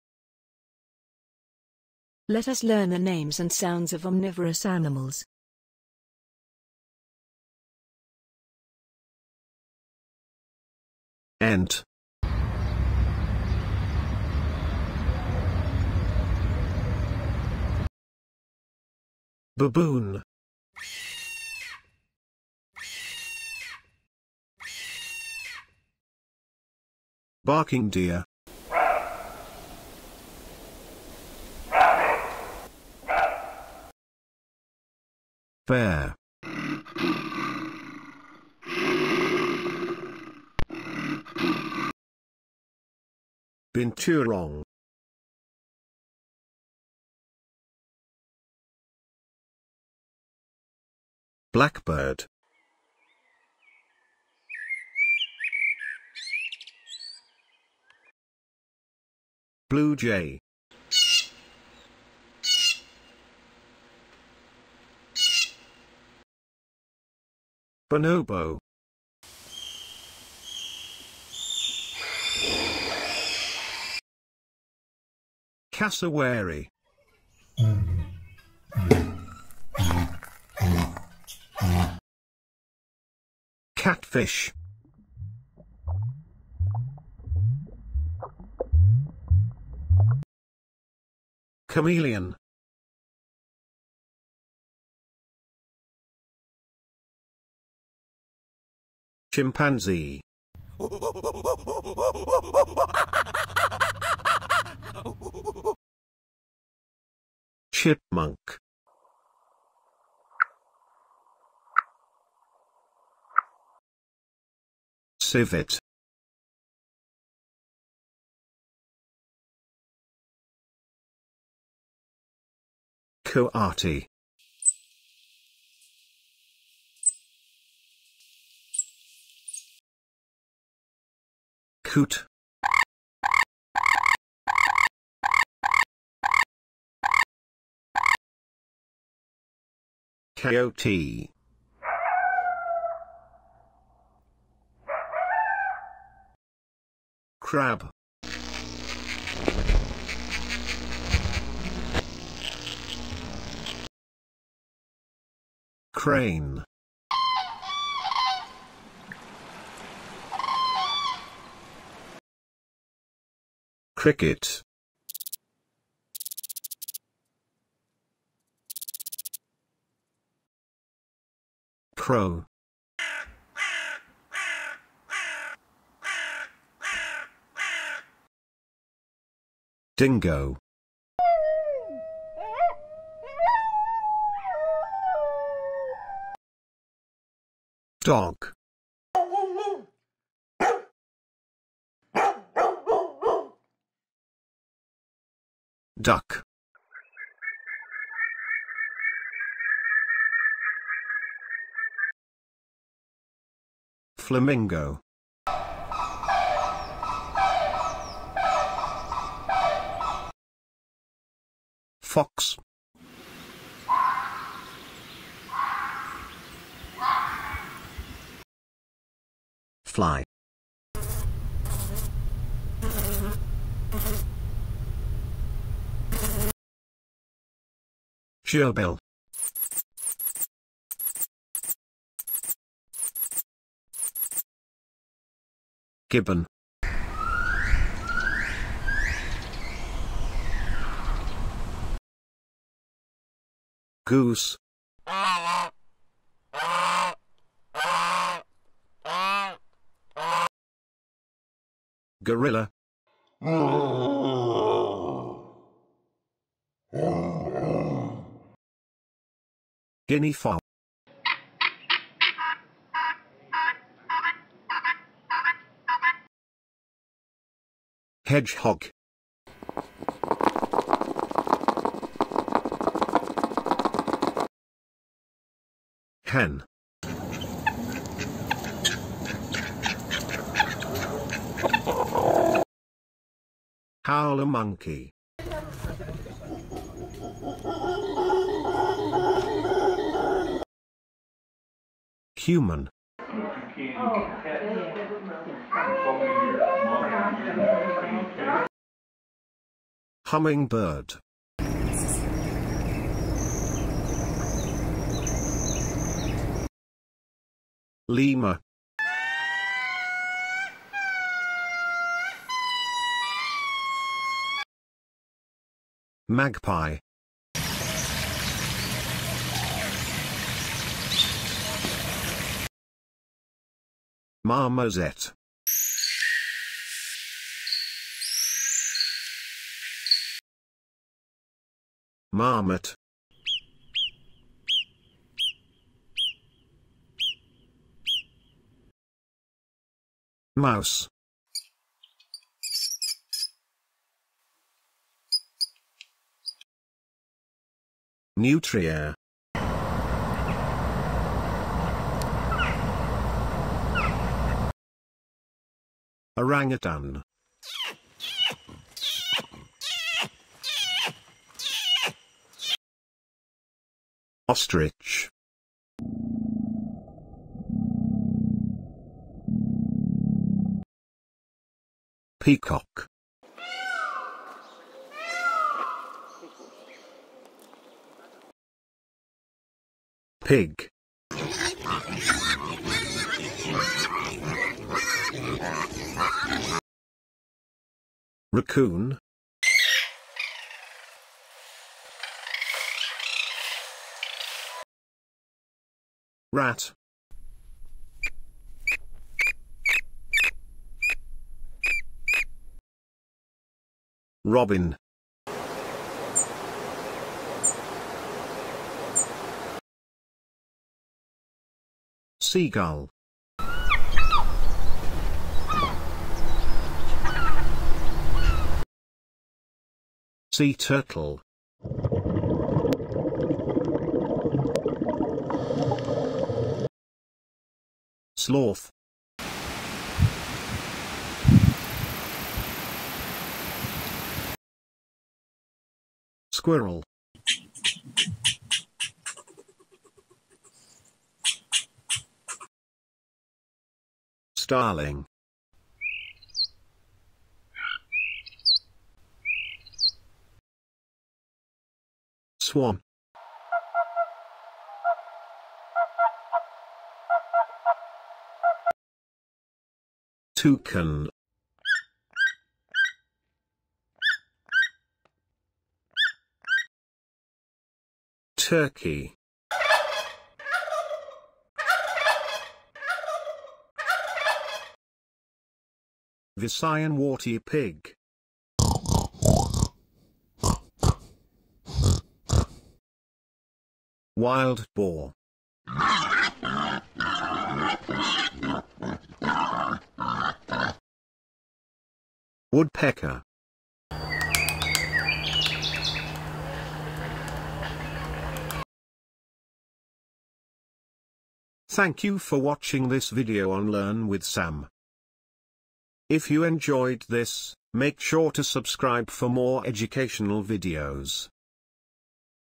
OMNIVOROUS ANIMALS : Names, Sounds and Call | Omnivores
This educational video will help you in learning omnivorous animals together with their sounds, names and vocalization in the English language. The video has words used in the English language to represent the noises and vocalizations of animals listed. However, only the most common vocalization is captured in this video. The animal may produce many other vocalizations as well.

An omnivore is an animal that is naturally able to eat both plants and meat, which may include eggs, insects, fungi, meat, and algae. In Latin, omnivore means to eat everything. Many omnivores evolved to their current state after several years and are opportunistic feeders. They rely on both vegetation and animal protein to remain healthy.

In this video, we have collated videos and photos of omnivores to teach omnivorous animal names, sounds and their vocalizations. 60 different animal names are presented with the correct British English pronunciation.

List of omnivorous animals: Ant, Baboon, Bear, Binturong, Black Bird, Blue Jay, Bonobo, Cassowary, Catfish, Chameleon, Chimpanzee, Chipmunk, Civet, Coati, Coot, Coyote, Crab, Crane, Cricket, Crow, Dingo, Dog, Duck, Flamingo, Fly, Fox, Gibbon, Goose, Gorilla, Guinea Fowl, Hedgehog, Hen, Howler Monkey, Humans, Humming Bird, Lemur, Magpie, Marmoset Monkey, Marmots, Mouse, Muntjac(Barking Deer), Nutria, Orangutan, Ostrich, Peacock, Pig, Raccoon, Rat, Robin, Sea Gull, Sea Turtle, Sloth, Squirrel, Starling, Swan, Toucan, Turkey, Visayan warty pig, Wild Boar, Woodpecker